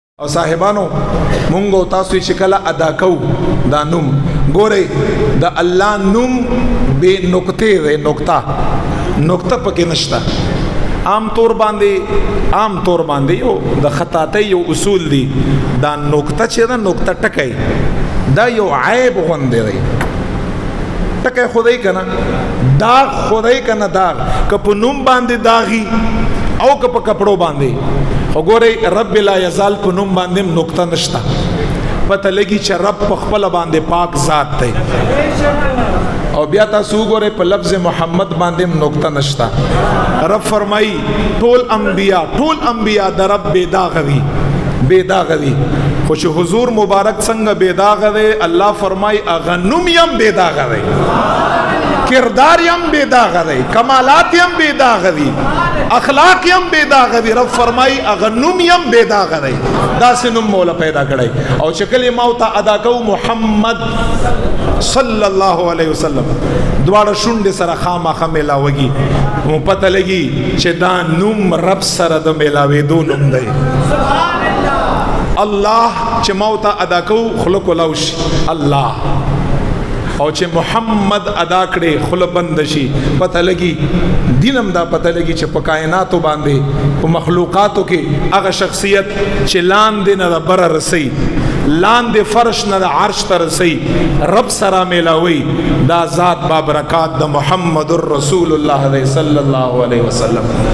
محمد صلی اللّٰہ علیہ وسلم کا نام بے نقطے کیوں ہے مفتی نقیب اللّٰہ جنیدی
بیان موضوع :نام محمدﷺ کی فضائلِ و خصائص

Location: Jamia Masjid, Darbar Shareef Hazrat Sheikh Junaid Peshawari RA